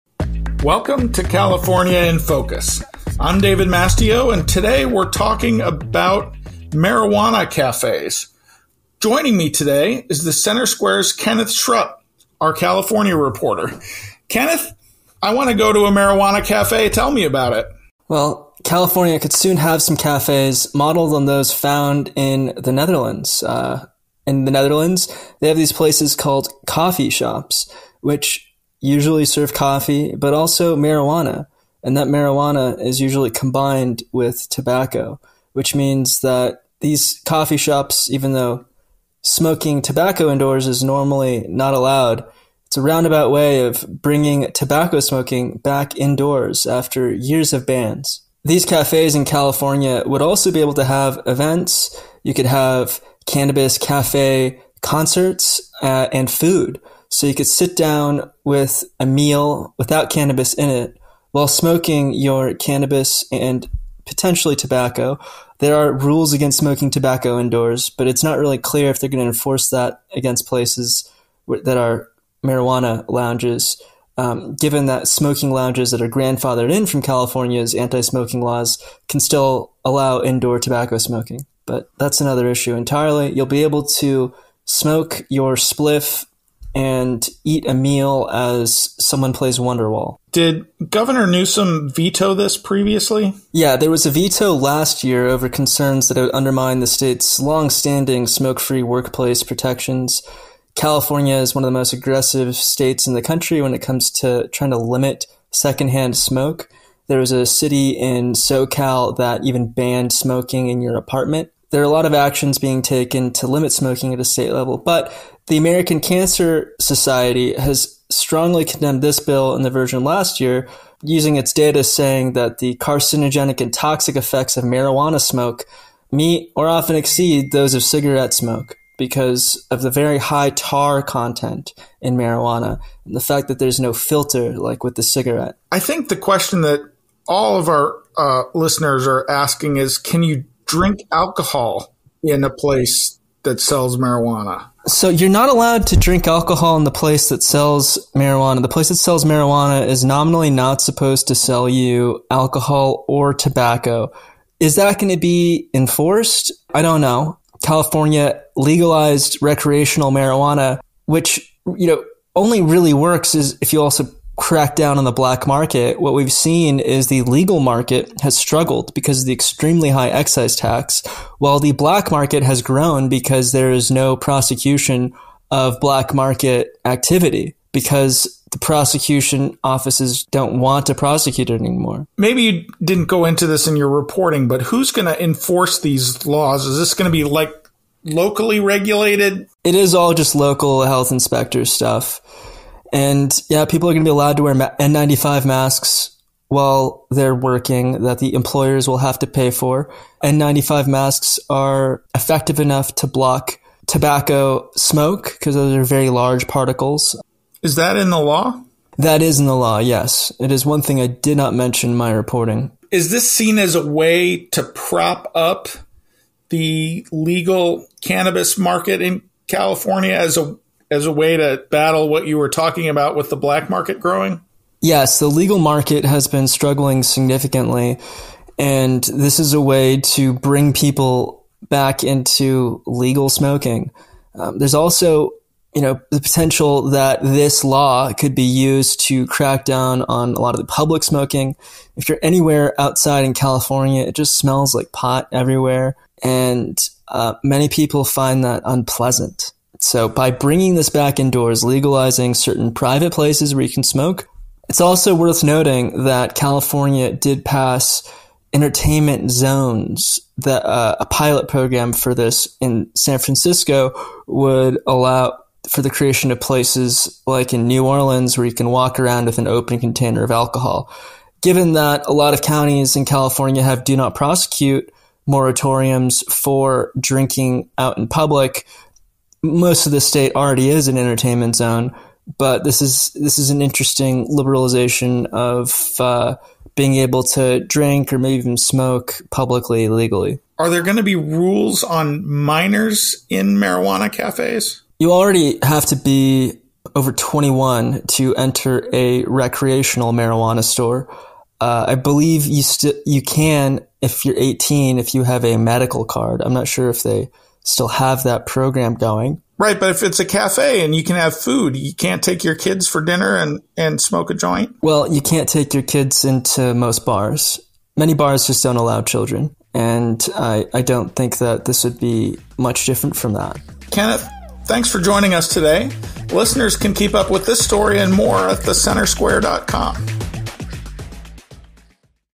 Caifornia’s Newsom Reverses Course to Allow Nation’s First Cannabis Cafes
Gov. Gavin Newsom signed a bill legalizing "cannabis cafes" and allowing commercial events there. The American Cancer Society condemned the law, noting secondhand marijuana smoke is often just as bad, if not worse, than cigarette smoke. California banned indoor smoking at entertainment venues in 1998, making this bill a major reversal in state policy. It's unclear if the law will allow individuals to smoke marijuana mixed with tobacco, as is the custom in Amsterdam, the city whose  "coffeeshops" (cafes that sell cannabis and coffee) the bill references as a model.